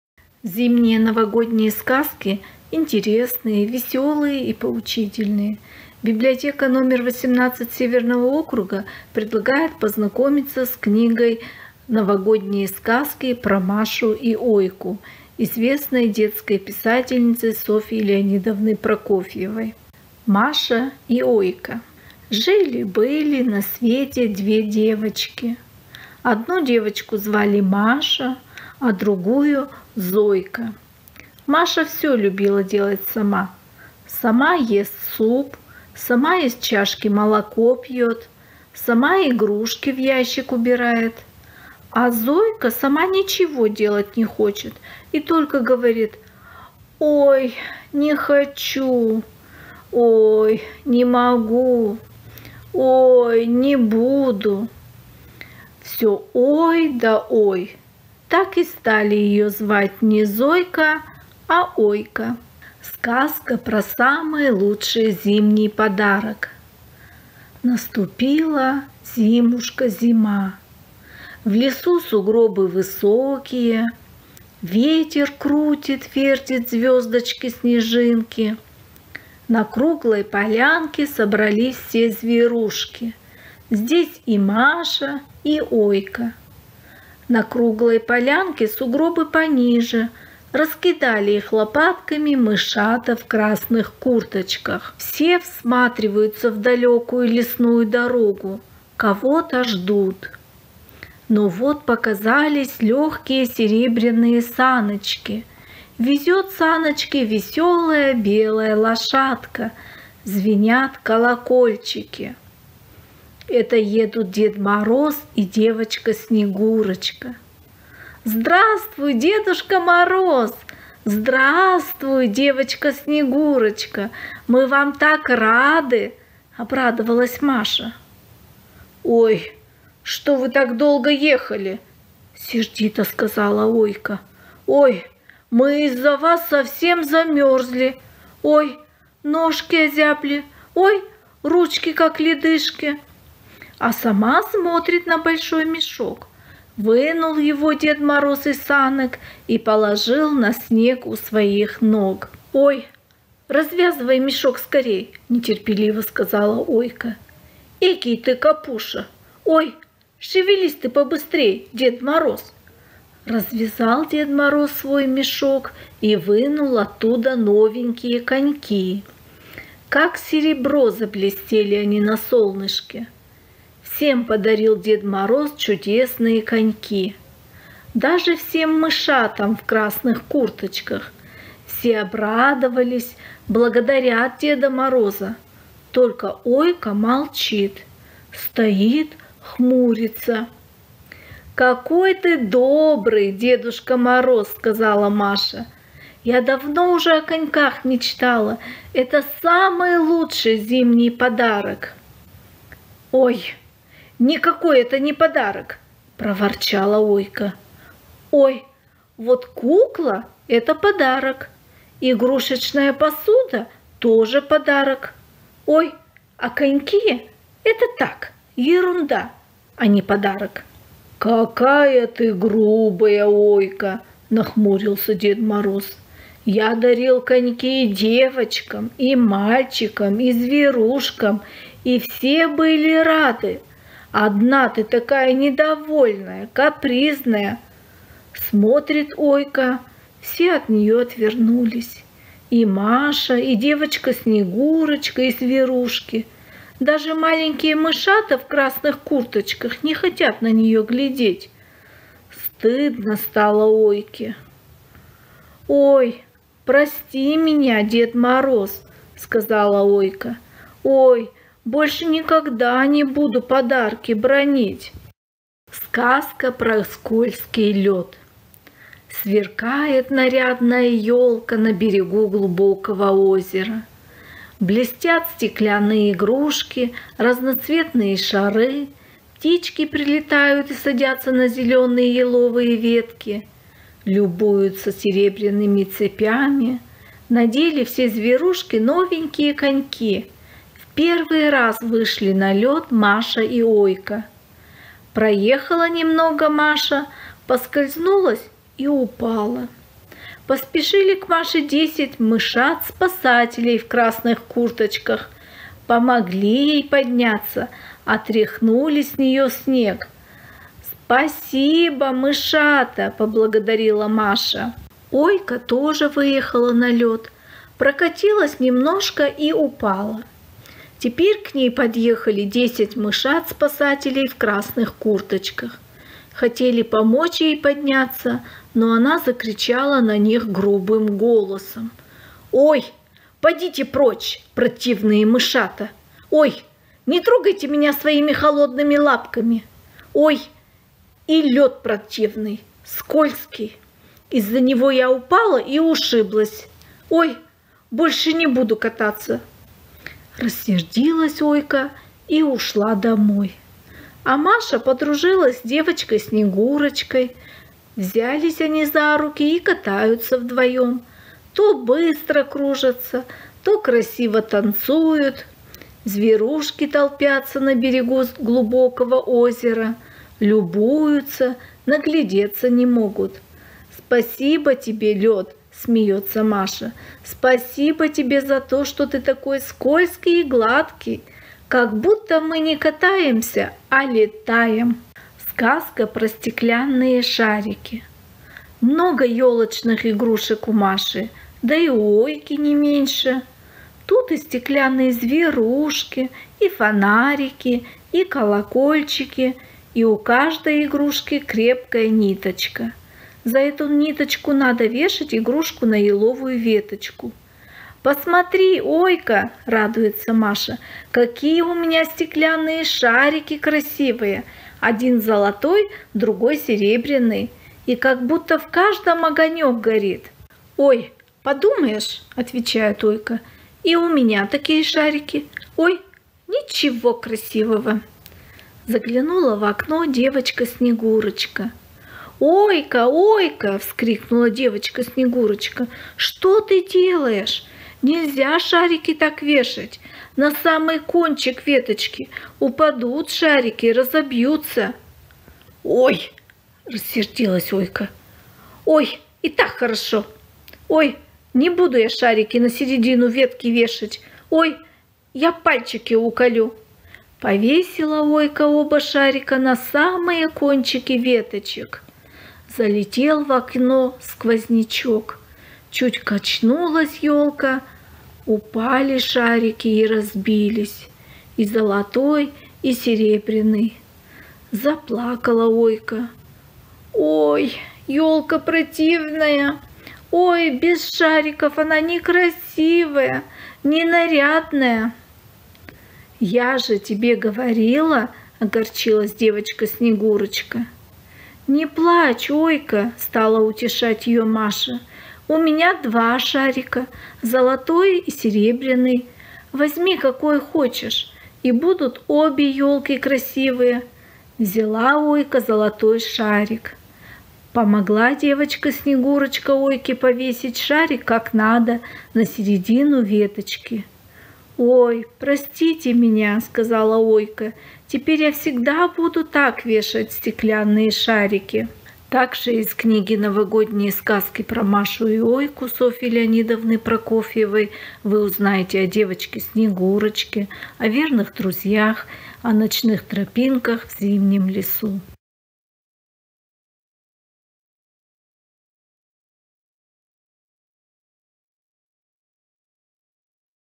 С. Прокофьева «Новогодние сказки про Машу и Ойку»
Библиотека № 18 Северного округа предлагает познакомиться с книгой Софьи Прокофьевой «Новогодние сказки про Машу и Ойку», в которой рассказывается про двух неразлучных подружек - упрямую, непослушную девочку Зойку, которая все время ойкала, и Машу - девочку добрую, умную и рассудительную.